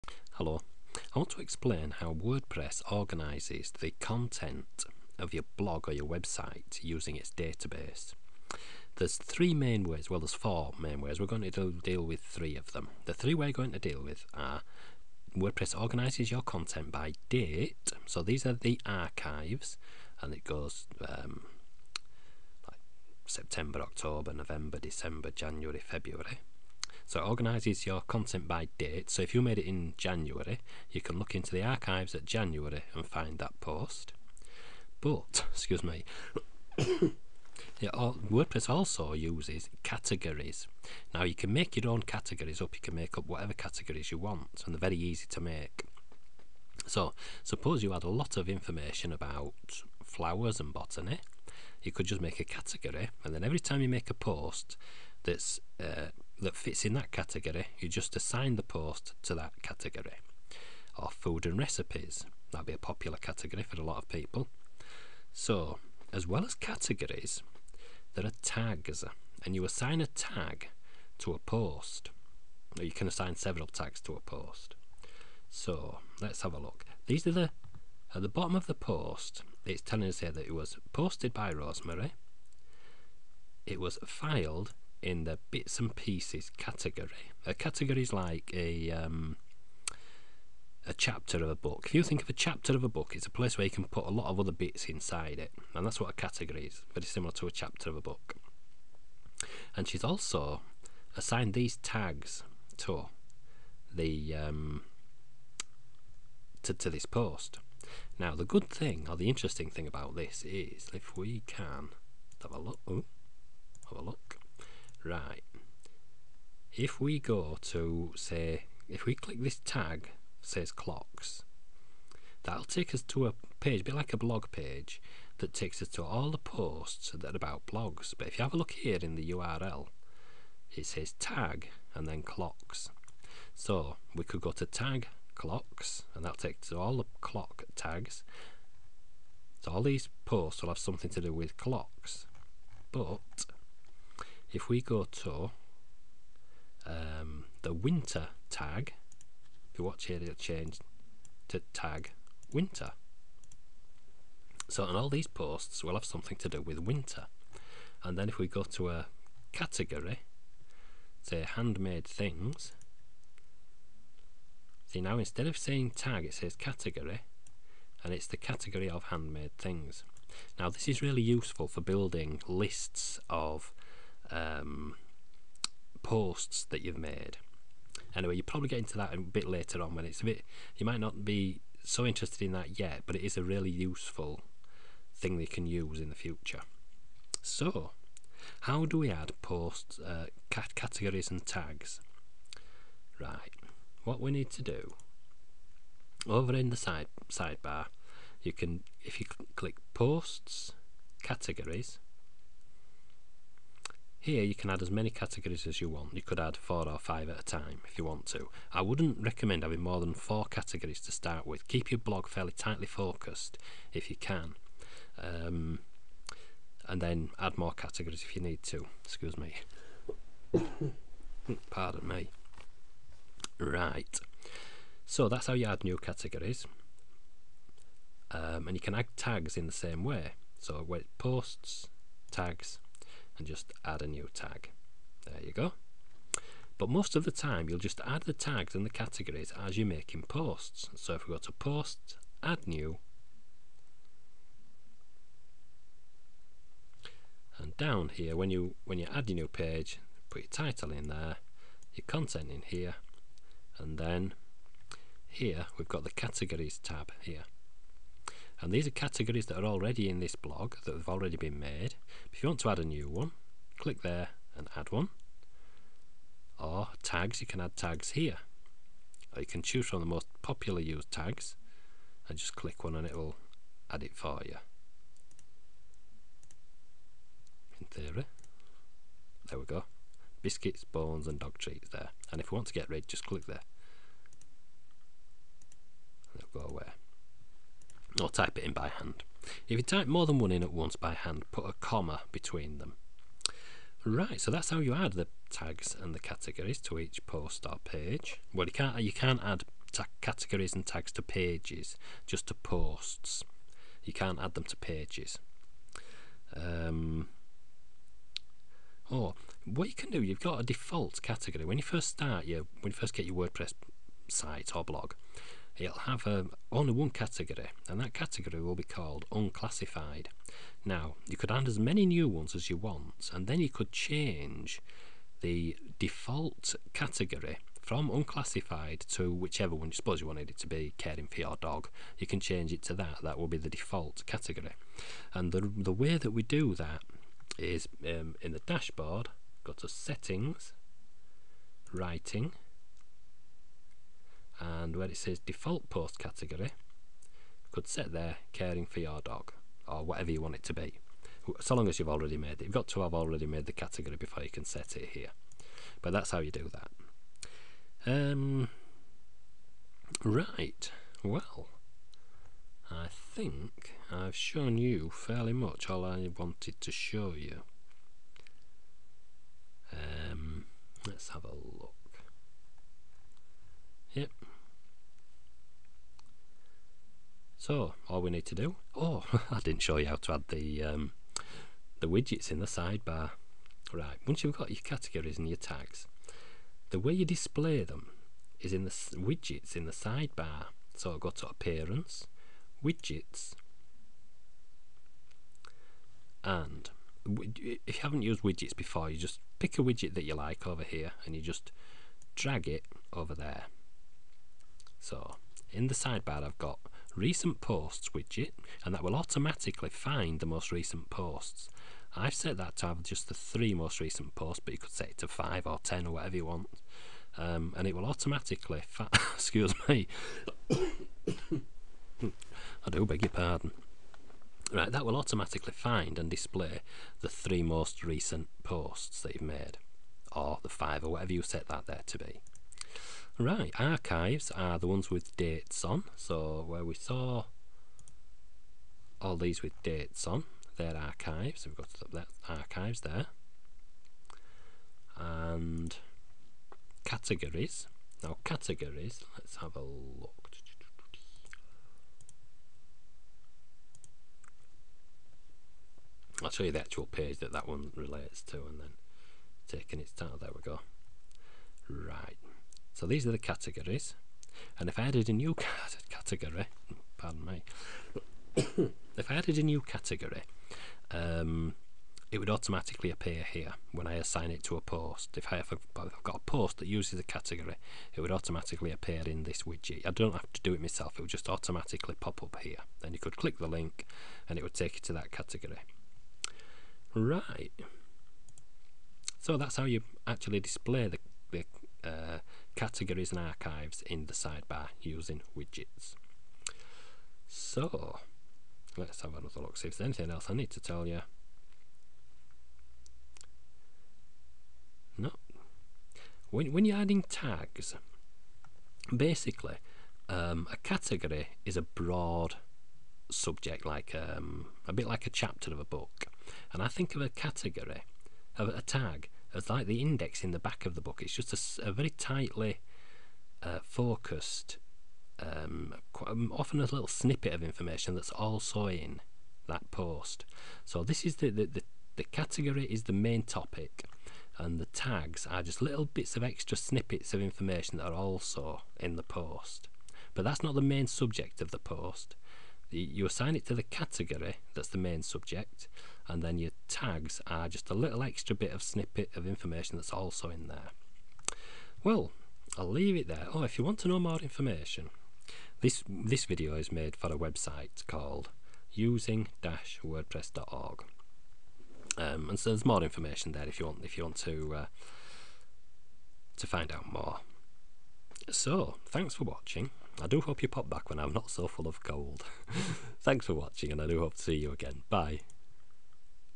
Categories and tags in WordPress, the basics
Learn how to add new categories and tags, assign them to a post and how to choose a new default category for your posts.